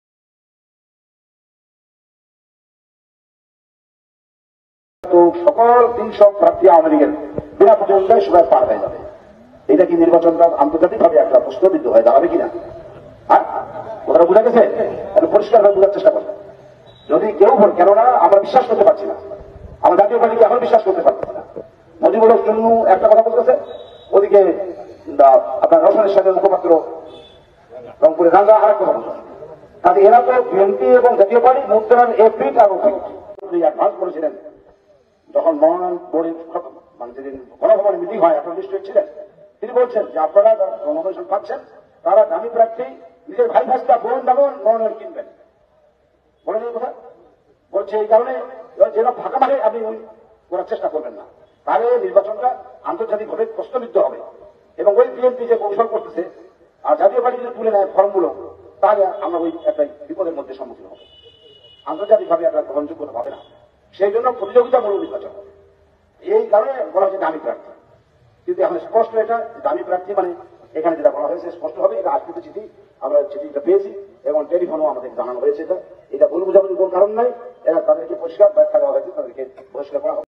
আওয়ামী লীগ মনোনীত প্রার্থী আফতাব উদ্দিন এমপির মুখে নির্বাচনের ডামি প্রার্থী নিয়ে সত্যতা স্বীকার।
নীলফামারী ১ আসনের আওয়ামী লীগ মনোনীত প্রার্থী আফতাব উদ্দিন সরকার এমপির মুখে নির্বাচনের ডামি প্রার্থী নিয়ে সত্যতা স্বীকার।

"নির্বাচনে ৩০০ এমপি আওয়ামী লীগের হবে, এটা কি আন্তর্জাতিক ভাবে স্বীকৃত হবে, হবে না। জাতীয় পার্টির উপর ও ভরসা পাচ্ছেন না প্রধানমন্ত্রী। গণভবনে প্রধানমন্ত্রী মনোনয়ন নিয়ে নিজেই বলেছেন যে আপনারা আপনাদের ভাই- বোনদের  ডামি প্রার্থী হিসেবে দাড়িয়ে দিবেন যাতে মনে হয় নির্বাচন সুষ্ঠু হচ্ছে।